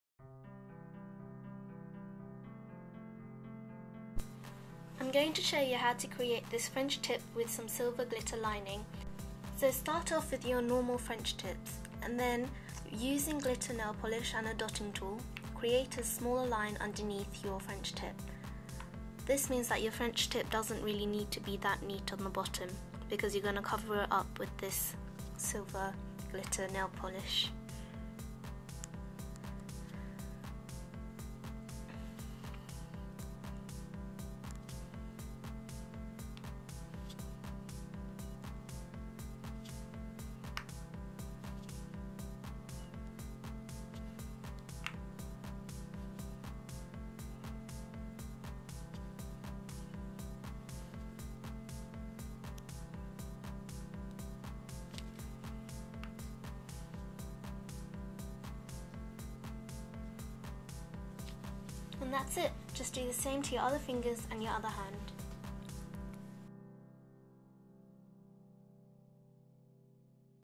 French Tip with Glitter Lining Tutorial
A tutorial on how to line your french tip with glitter.